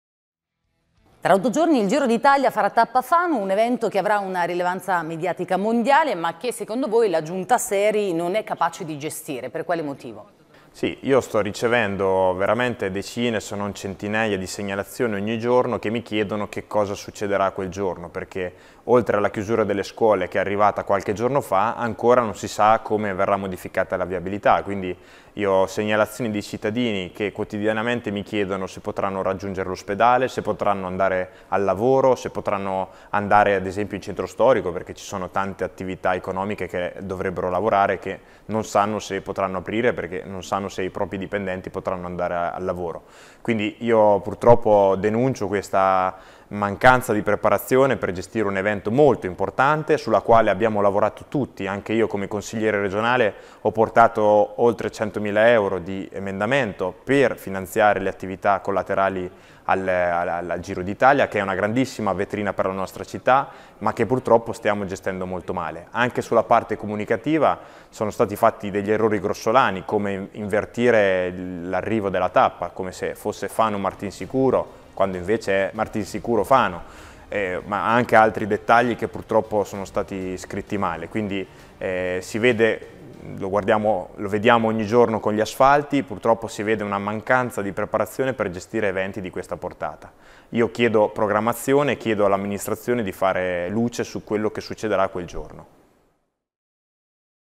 Serfilippi: “Giunta Seri incapace di gestire il Giro d'Italia”
“Per il Giro d’Italia, l’amministrazione ancora deve informare la città sulle modifiche alla viabilità e di fatto paralizza migliaia di famiglie e cittadini”. Lo dichiara la coalizione Per Fano, secondo la quale è “inaccettabile la superficialità con cui l'amministrazione sta gestendo la parte organizzativa dell’arrivo della Tappa della Corsa in rosa prevista per il 16 maggio”.